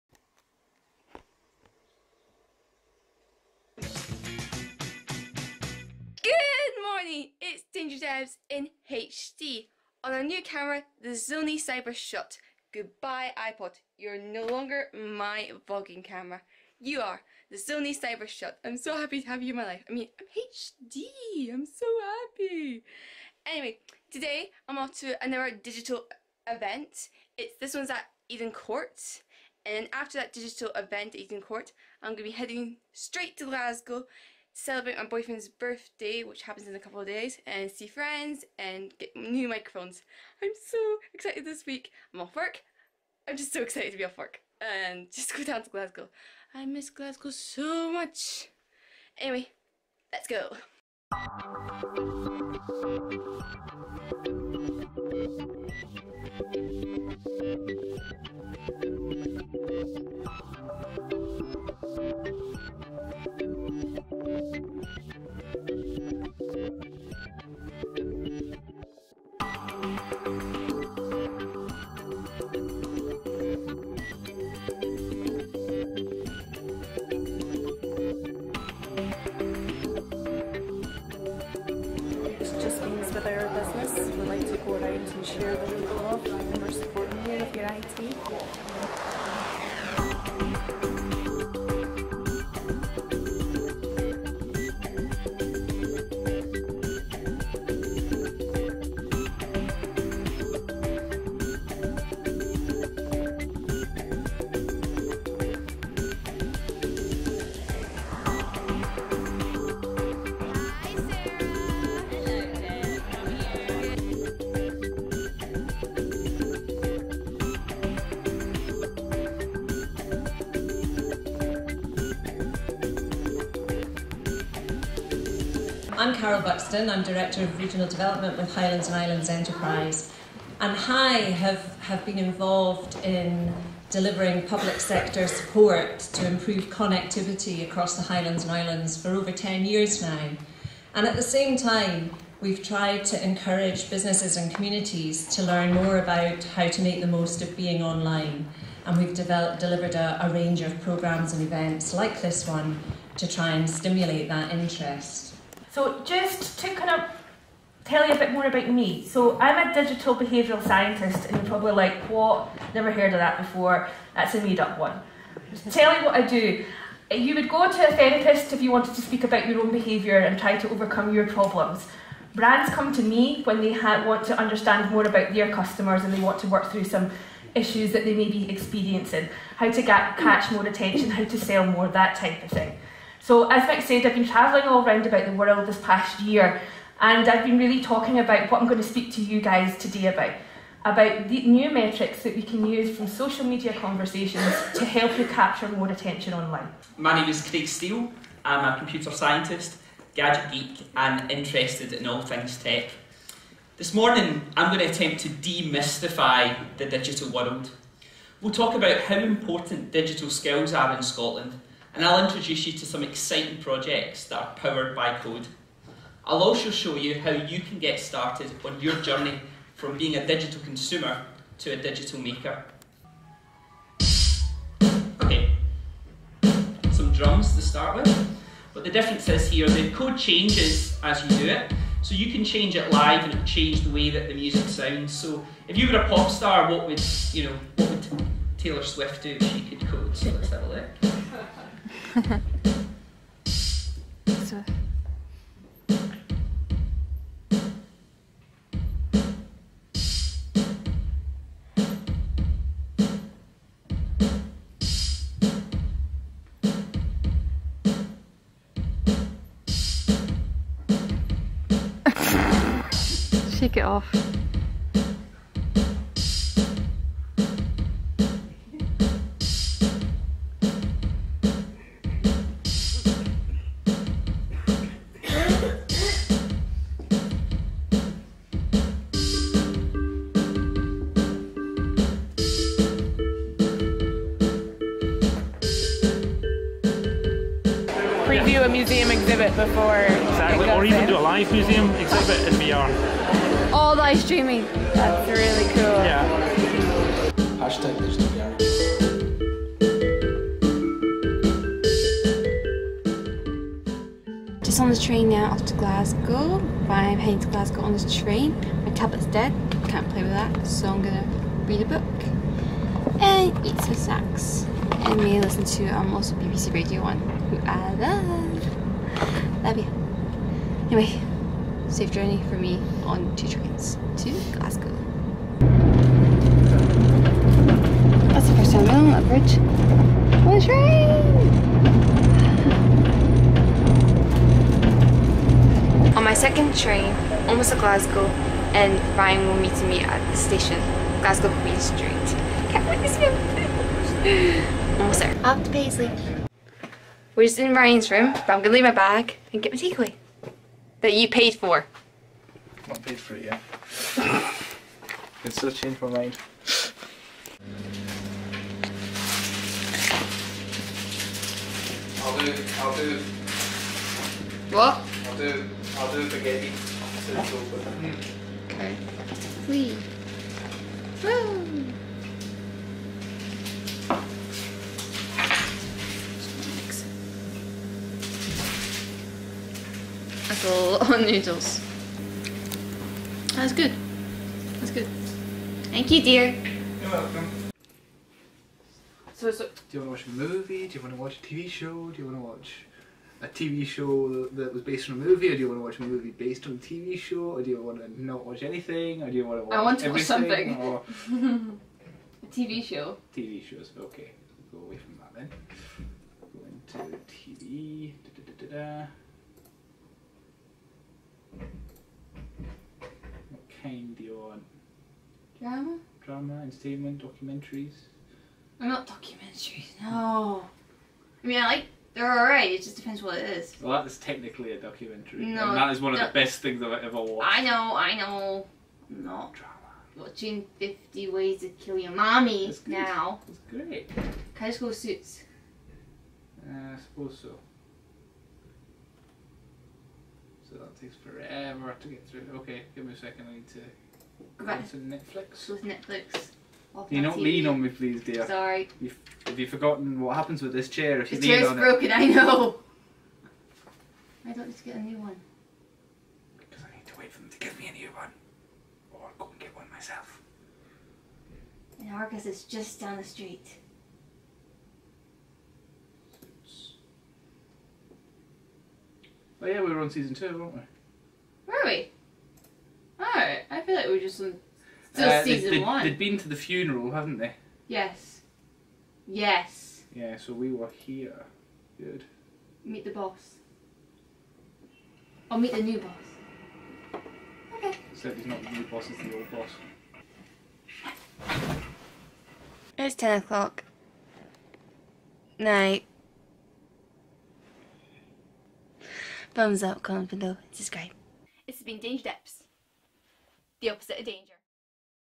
Dangerdebbs at hellodigital event | Vlog 11
Capturing the future of tech, marking and coding at hellodigitial extra17 at Eden Court Theatre, hosted by highlands and Islands enterprise. Also heading to Glasgow for my Ryan's birthday in a few weeks (boyfriend)

Featuring business at the event:
Highlands and Islands enterprise
Digital Scotland
Logo
CoderjodoScotland
CityFibe
Ecosse
Dell - Cyber Security
Microsoft
Bing
Google garage - Google
dp digital media
Capital Document Solution
Highlands Office Equipment
Krisp Camp Security
Yellow cherry
Exclamation Marketing
Sublime

Featuring people in the video
Carroll Buxton - Director of Regional Development
Dr Jillan Ney -
Craig Steele - Computer Scientist
Andrew Macdonald
Sarah S
Ryan Metcaft

Sorry this video is a couple of days late. Due to on holiday

You can may watch, comment and SHARE again and again. I love keep broadcasting, work with children and love one another :). Be apart of the huge journey.